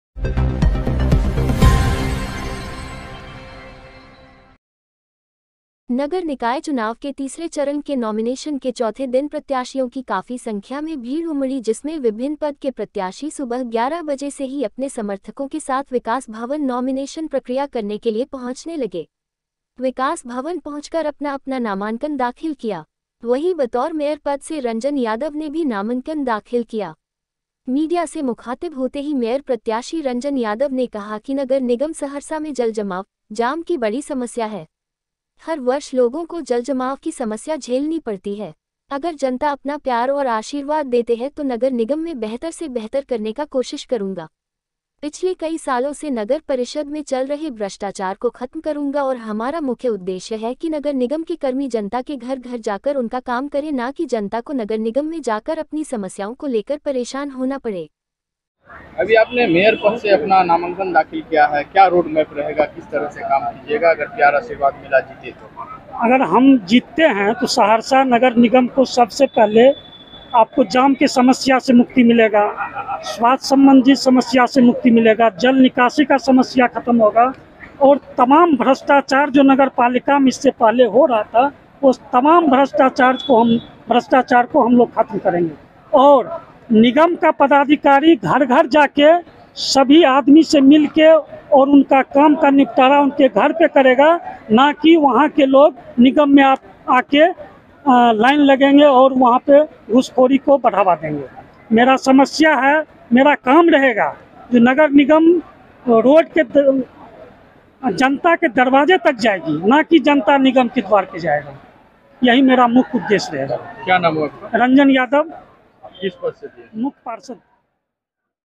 नगर निगम के कर्मी जनता के घर-घर जाकर करें काम समस्या को लेकर जनता को ना होना पड़े परेशान: रंजन यादव
हमारा उद्देश्य नगर निगम के कर्मी जनता के घर-घर जाकर करें समस्या को दूर : रंजन यादव ।
नगर निकाय चुनाव के तीसरे चरण के नॉमिनेशन के चौथे दिन प्रत्याशियों की काफी संख्या में भीड़ उमड़ी जिसमें विभिन्न पद के प्रत्याशी सुबह 11:00 से ही अपने समर्थकों के साथ विकास भवन नॉमिनेशन प्रक्रिया करने के लिए पहुंचने लगे। विकास भवन पहुंचकर अपना-अपना नामांकन दाखिल किया।वहीं बतौर मेयर पद से रंजन यादव ने भी नामंकन दाखिल किया।मीडिया से मुखातिब होते ही मेयर प्रत्याशी रंजन यादव ने कहा की नगर निगम सहरसा में जलजमाव, जाम की बड़ी समस्या है।हर वर्ष लोगों को जलजमाव की समस्या झेलनी पड़ती है।अगर जनता अपना प्यार और आशीर्वाद देते है तो नगर निगम में बेहतर से बेहतर करने का कोशिश करूँगा।पिछलें कई सालों से नगर परिषद में चल रहे भ्रष्टाचार को खत्म करूंगा और हमारा मुख्य उद्देश्य है की नगर निगम के कर्मी जनता के घर-घर जाकर उनका काम करें ना कि जनता को नगर निगम में जाकर अपनी समस्याओं को लेकर परेशान होना पड़े।